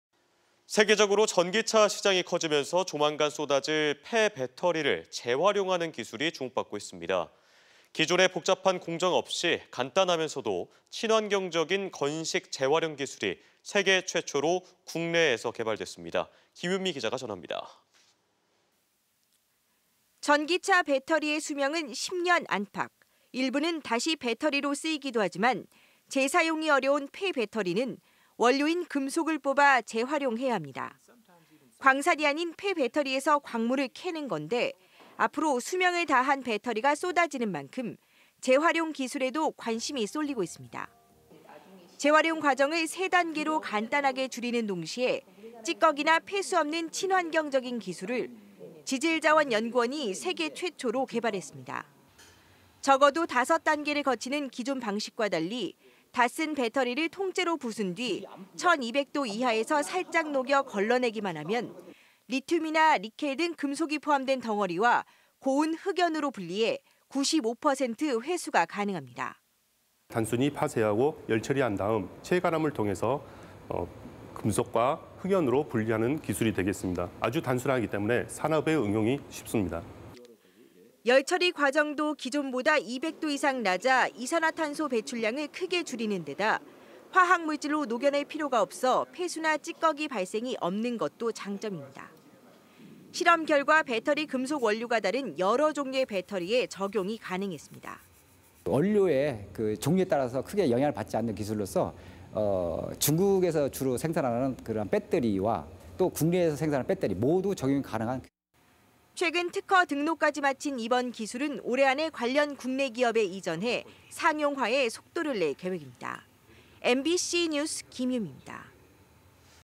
폐배터리도 'OK' 재활용 기술 등장/대전MBC
"전기차 시장이 커지면서 수명을 다한 폐배터리가 앞으로 쏟아질 전망입니다. 기존보다 간단하면서도 친환경적인 재활용 기술이 세계 최초로 국내에서 개발됐습니다"